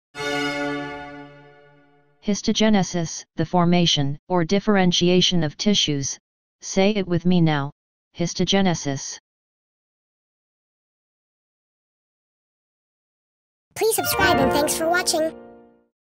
histogenesis - Smart & Obscure English Words Defined 👁️🔊🗣🧠✅
This video is about the word "histogenesis". Here's a video of some of the most obscure English words with definitions that will definitely make you sound smart (more smarter) if used properly.